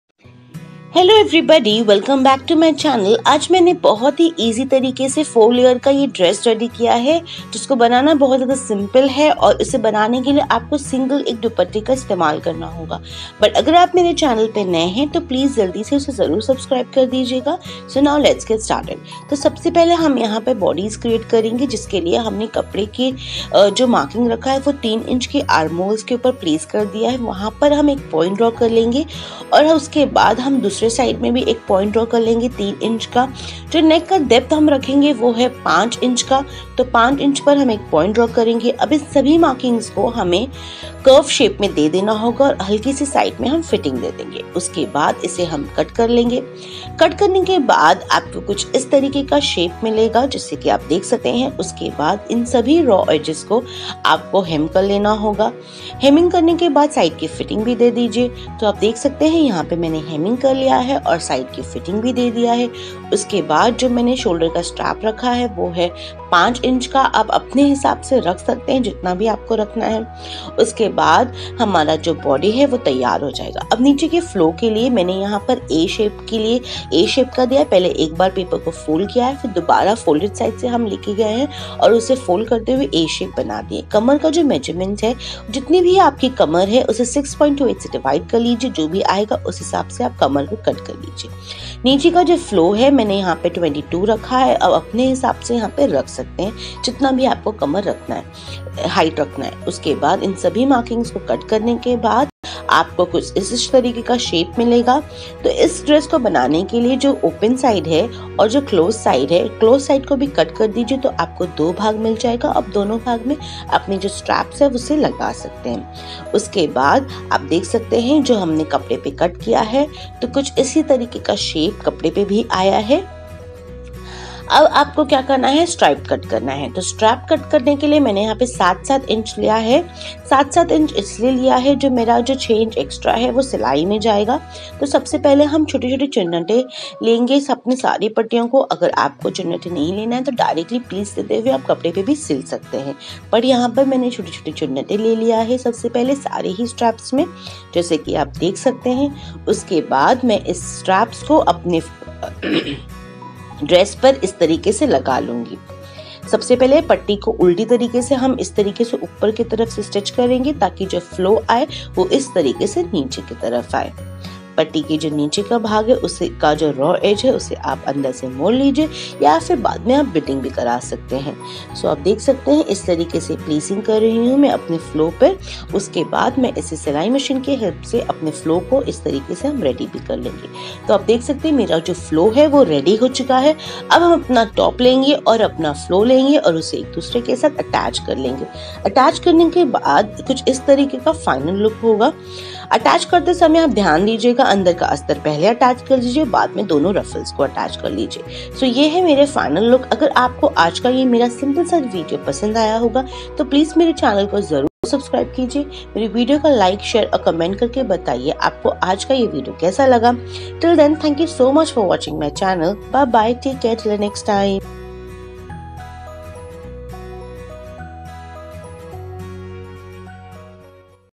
Diy old duppatta into 4 layer frill dress/diy frill dress/ summer dress
Today Im gonna show you how you can convert a old saree / fabric  long Frill  gown easily in 10 minutes.

So today im gonna show easy method on creating saree into three layer Frill gown

I  hope that you liked this DIY video.